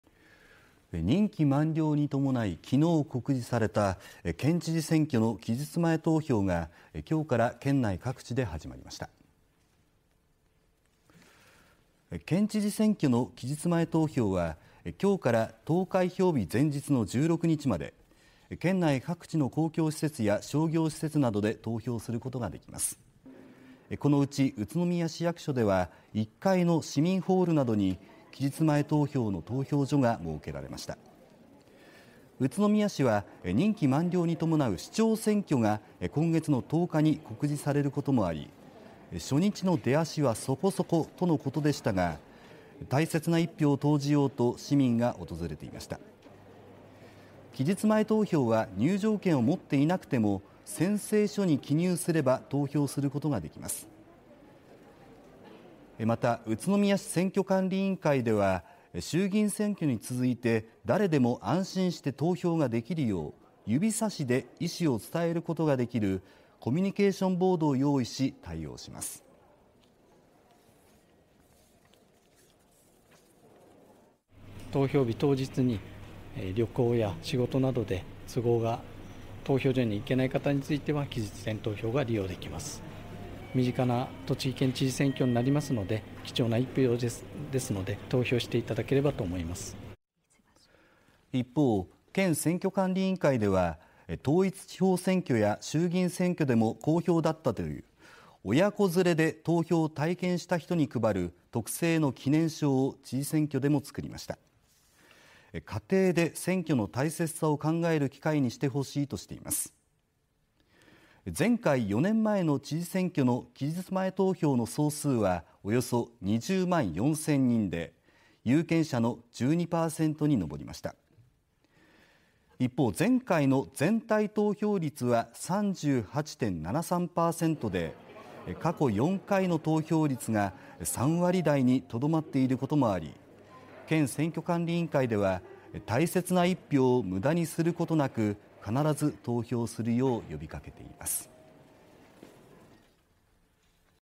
栃木県知事選挙 期日前投票始まる
任期満了に伴い３１日告示された知事選挙の期日前投票が１日から県内各地で始まりました。

知事選挙の期日前投票は、１日から投開票日前日の１６日まで県内各地の公共施設や商業施設などで投票することができます。

このうち宇都宮市役所では１階の市民ホールに期日前投票の投票所が設けられました。

宇都宮市は任期満了に伴う市長選挙が１１月１０日に告示されることもあり「初日の出足はそこそこ」とのことでしたが、大切な一票を投じようと市民が訪れていました。

期日前投票は入場券を持っていなくても宣誓書に記入すれば投票することができます。

また、宇都宮市選挙管理委員会では衆議院選挙に続いて誰でも安心して投票ができるよう指差しで意思を伝えることができるコミュニケーションボード」を用意し、対応します。

一方、県選挙管理委員会では統一地方選挙や衆議院選挙でも好評だったという、親子連れで投票を体験した人に配る特製の「記念証」を知事選挙でも作りました。

家庭で選挙の大切さを考える機会にしてほしいとしています。

前回４年前の知事選挙の期日前投票の総数はおよそ２０万４千人で有権者の１２％に上りました。

一方、前回の全体投票率は「３８．７３％」で過去４回の投票率が３割台に留まっていることもあり、県選挙管理委員会では大切な一票を無駄にすることなく必ず投票するよう呼びかけています。